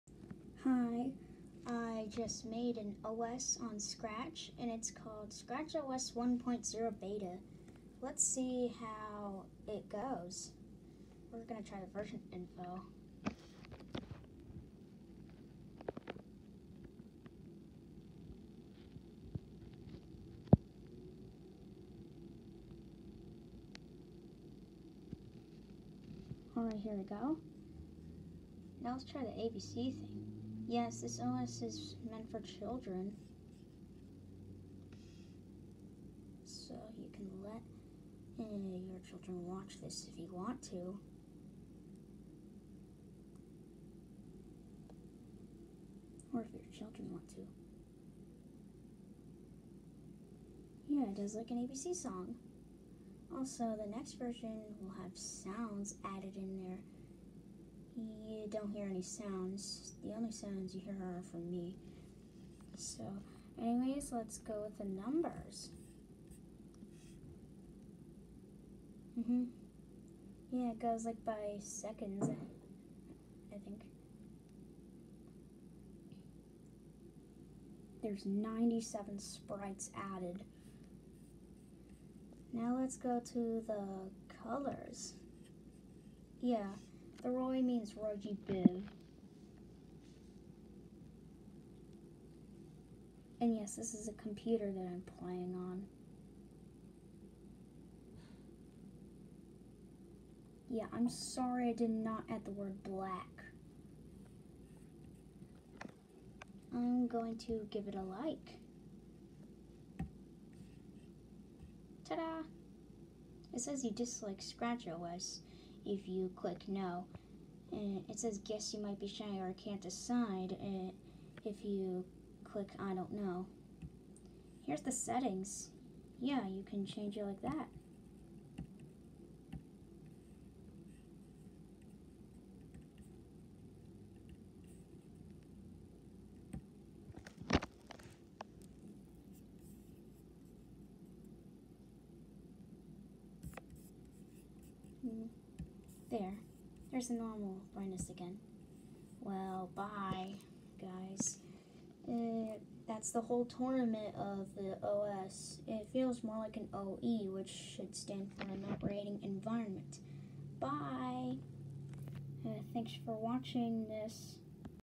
Scratch OS 1.0 Beta!
Sprites: 97 like I said
Meant for children? Yes!
Meant for learning? Yes!
It feels more like an operating environment (OE).
Is it made by me? Yes!
How long it took: About 2 weeks of work! I started working on this since 4th of July this year!
It can be based on Windows!